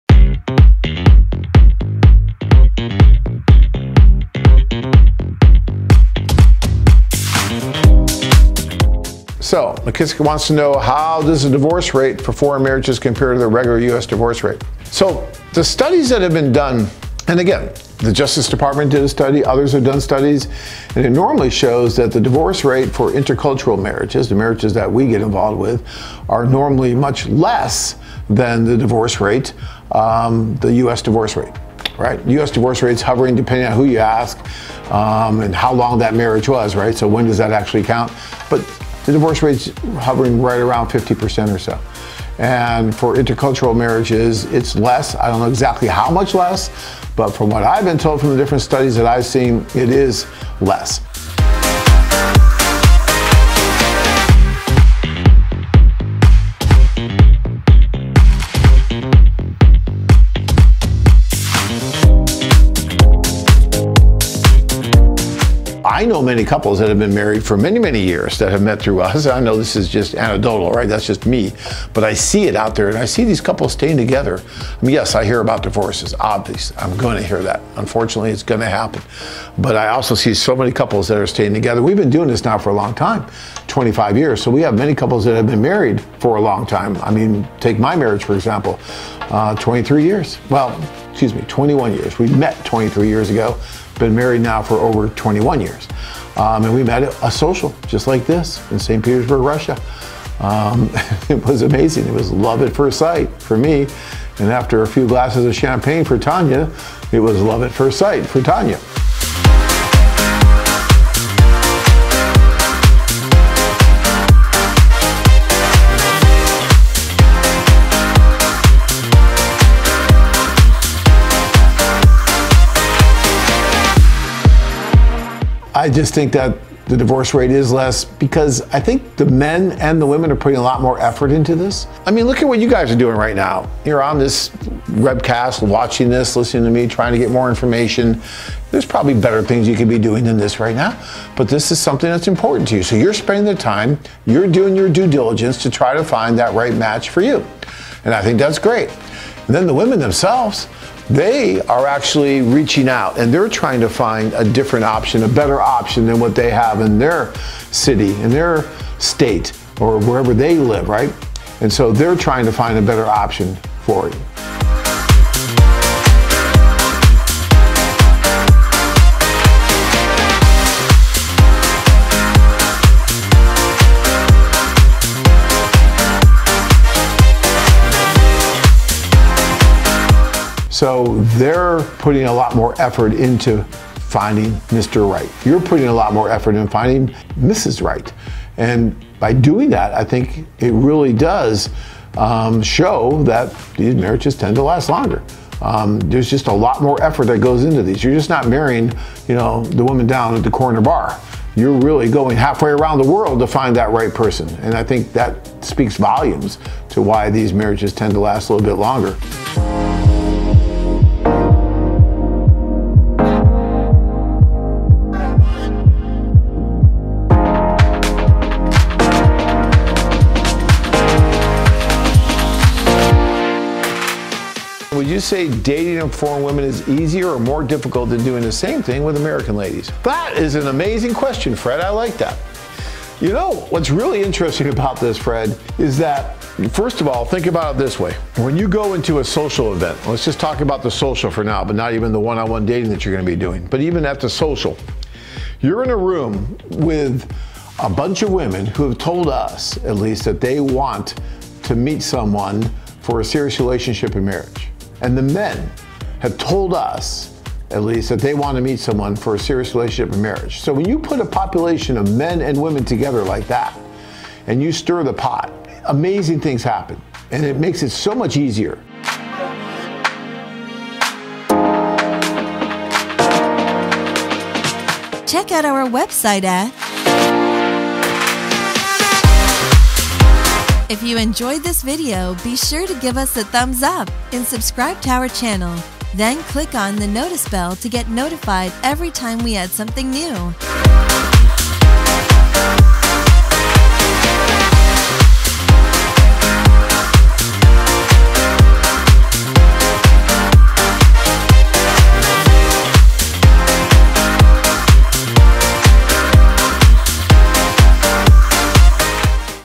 Do Marriages with Ukraine Women Have HIGH Divorce Rates?
Do you worry about the prevalence of breakups or divorce among interracial couples? Good news here - that’s one less thing you should worry about. Many have done research about this and found out that mixed couples like Ukraine women and western men tend to have a lower divorce rate than the regular US marriages.

Interracial marriage may pose different kinds of struggles but turns out, these only strengthen the bond of mixed race couples. This is very true when dating and marrying Eastern European women. Many men can even attest to it how many decades they have been married to their Ukrainian wives.

All of us want to meet the love of our lives without divorce factoring in. Though being one of the interracial couples sound daunting given the possible languages and cultural differences, it’s one of the most lasting and satisfying kinds of relationships there are. This calls for you to meet Ukraine women now!